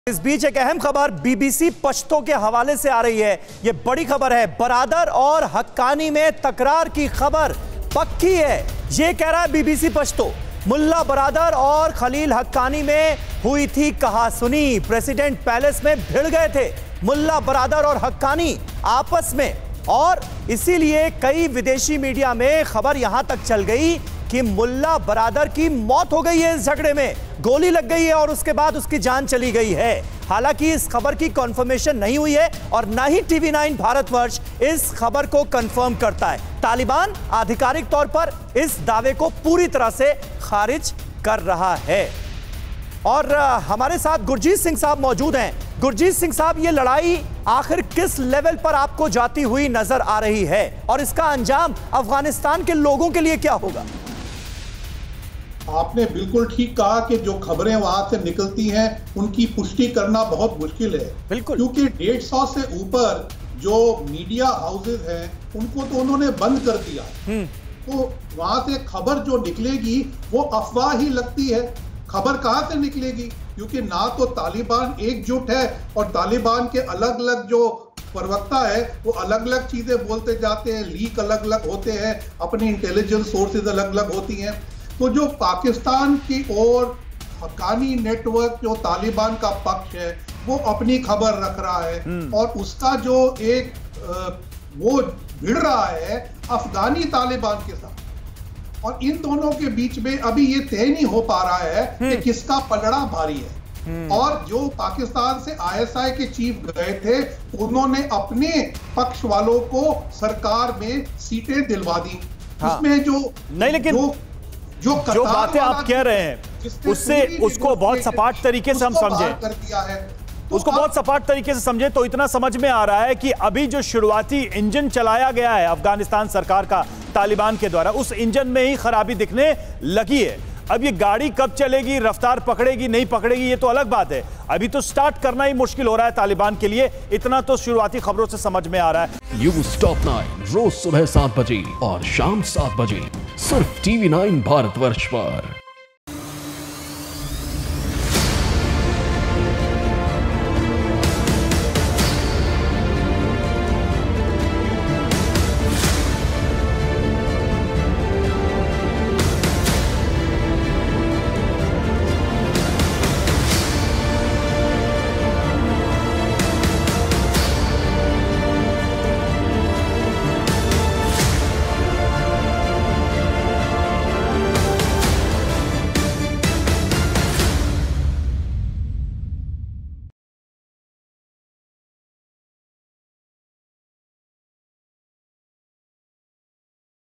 BBC पश्तो के हवाले से बहुत बड़ी खबर, Baradar और Haqqani में तकरार की खबर पक्की
BBC पश्तो के हवाले से बहुत बड़ी खबर सामने आई है. बरादर और हक्कानी में तकरार की खबर पक्की है. मुल्ला बरादर और खलील हक्कानी में कहासुनी हुई थी.

Credit: International । SamirAbbas । RaviMishra ।